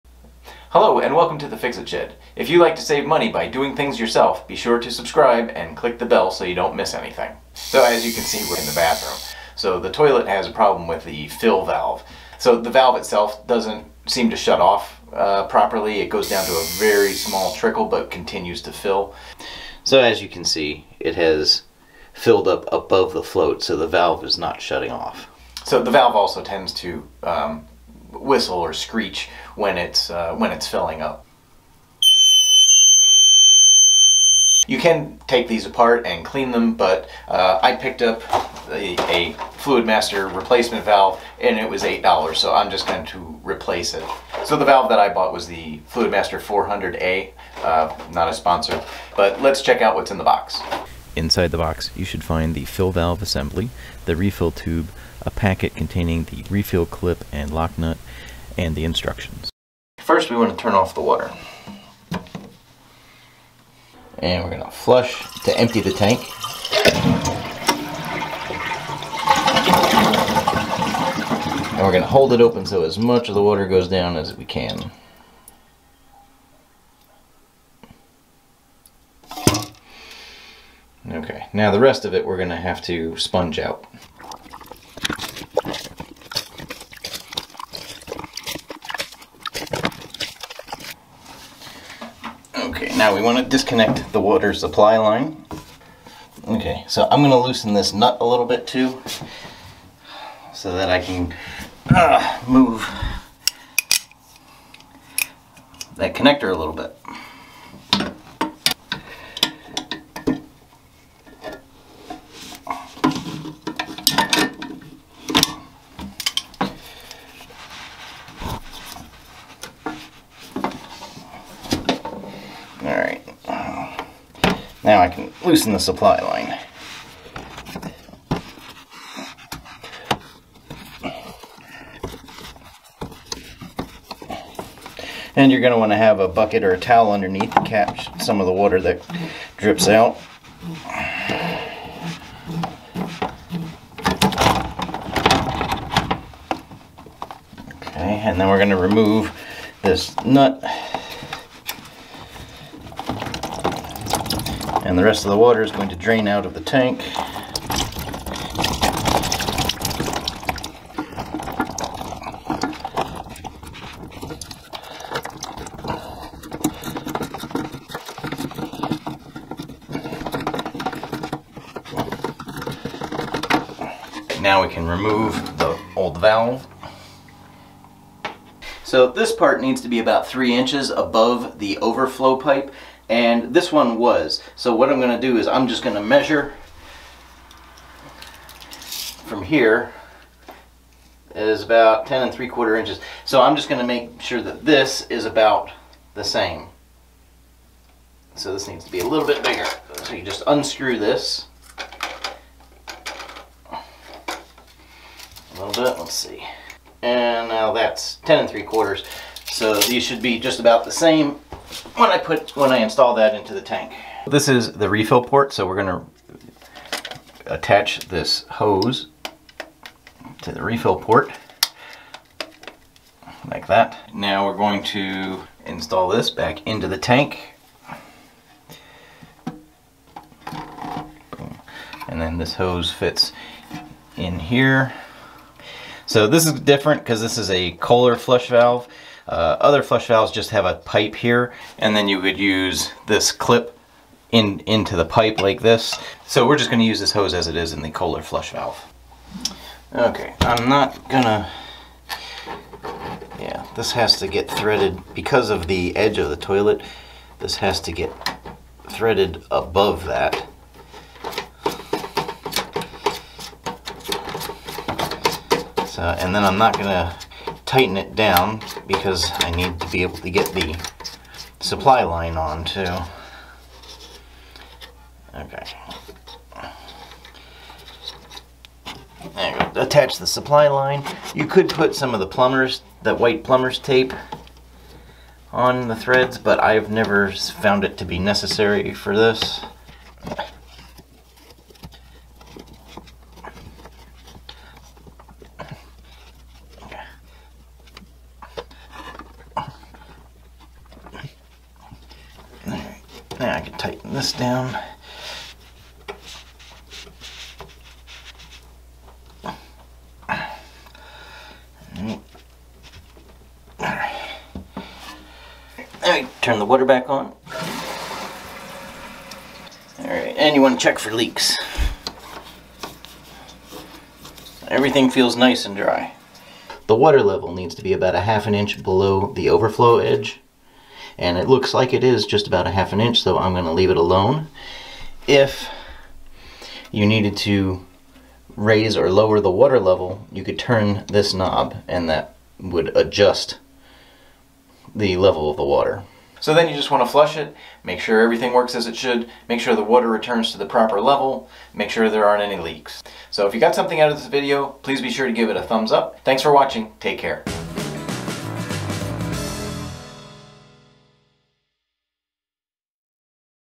Installing FluidMaster 400A Fill Valve in your toilet. Do It yourself!
There are lots of home repairs that you can do yourself.  Replacing the toilet fill valve is easy and can save you money.

Fluidmaster 400A (2 pack)
Fluidmaster 400CRP14 Fill Valve and Flapper
Fluidmaster 400AH PerforMAX

Inline filter